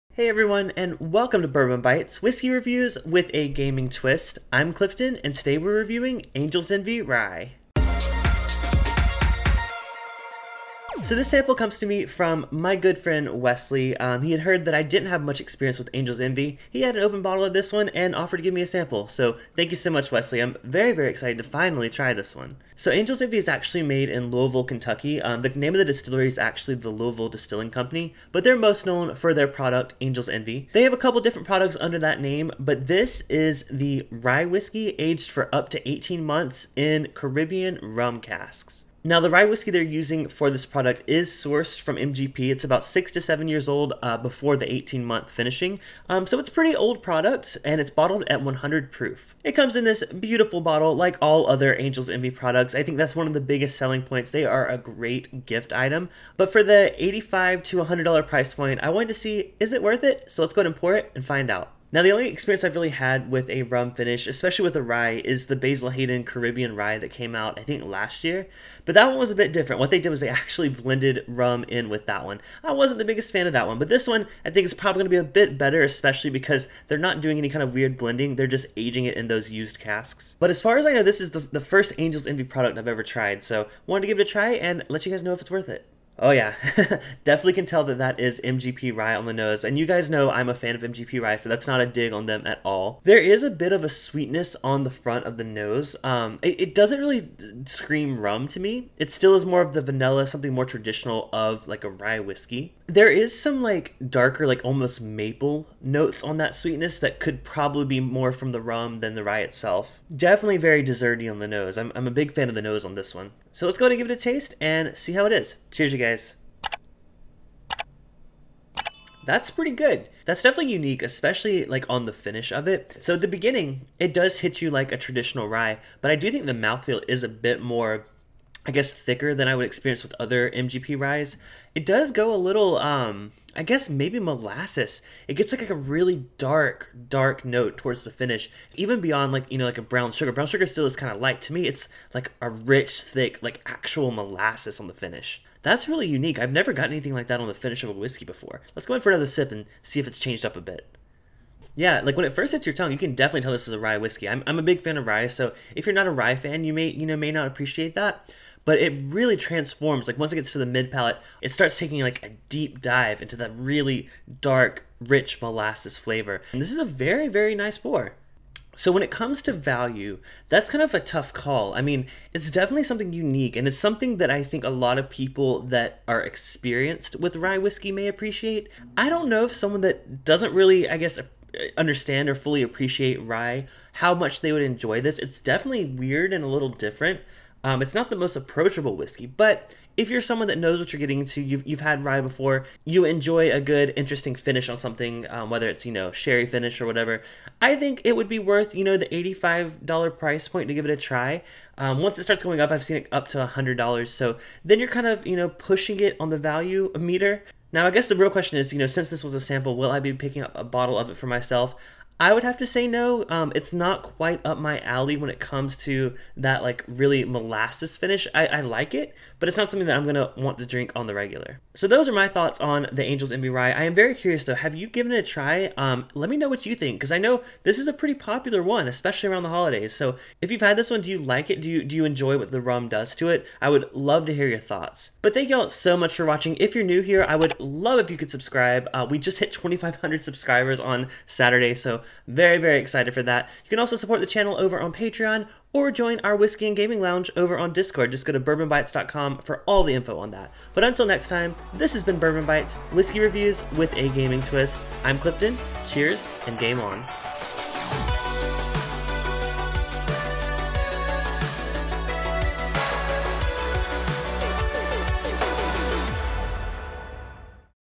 Angel's Envy Rye - Caribbean Rum Cask Finish (Review)
Trying Angel's Envy Rye which has been finished for up to 18 months in Caribbean Rum casks!